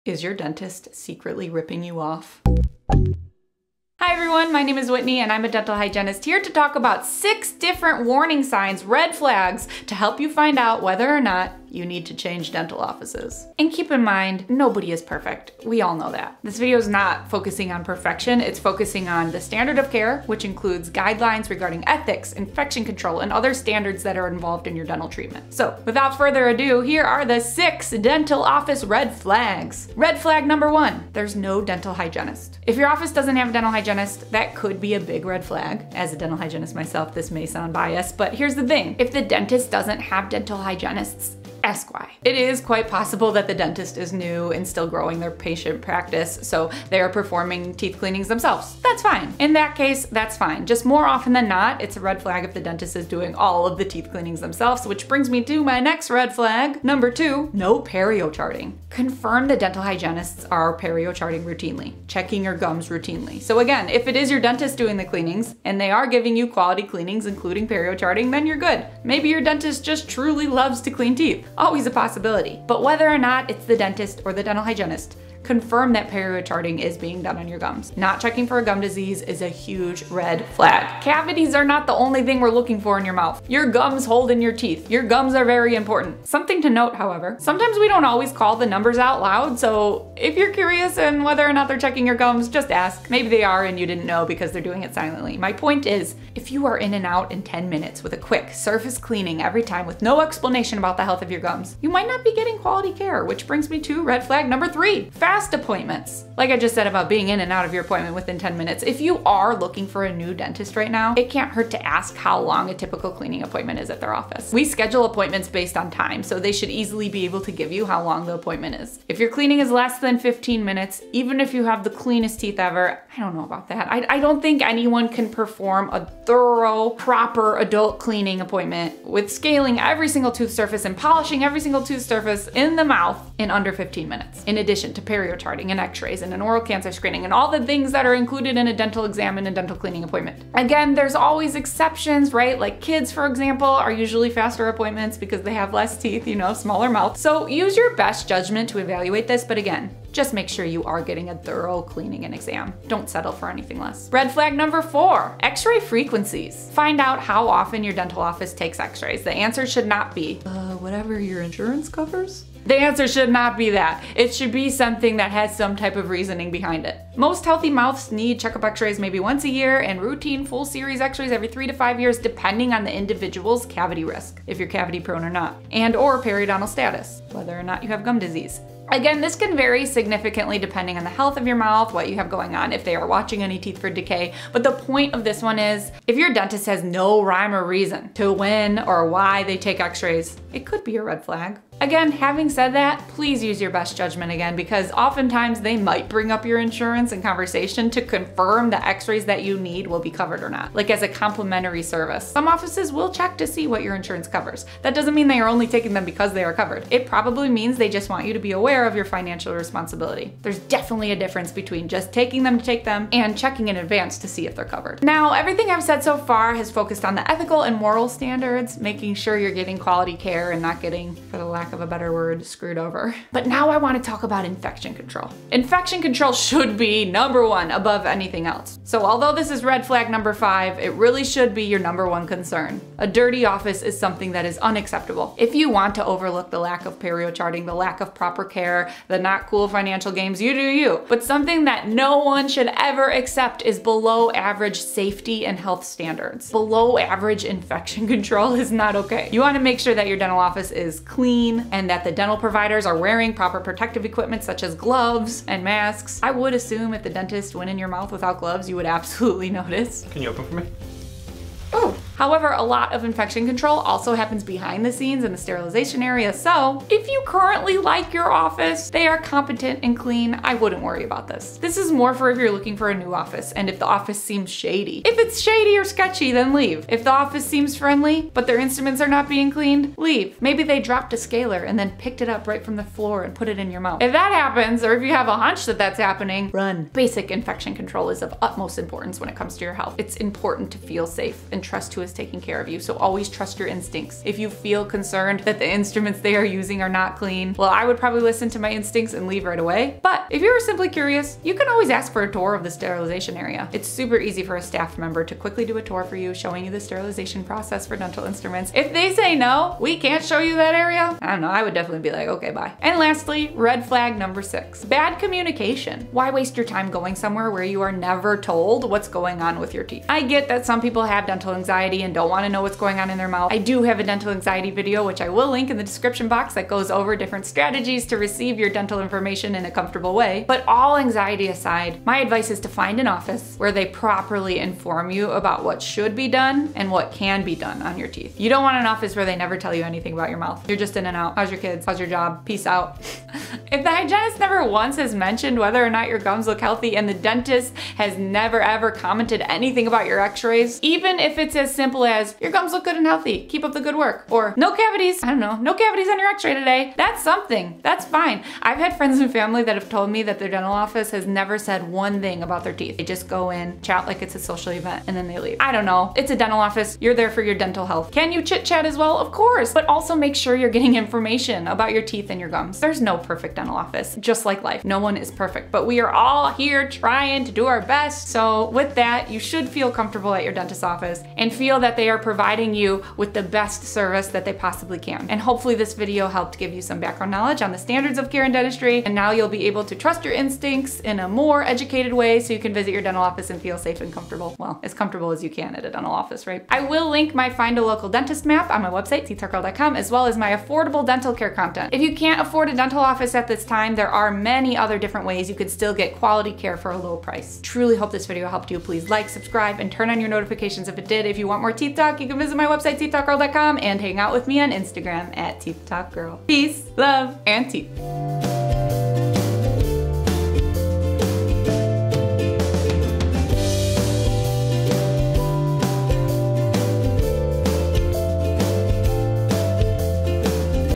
Dental Office RED FLAGS 🚩
Let's talk about how to spot out things to avoid when trying to find the right dentist for you and your family!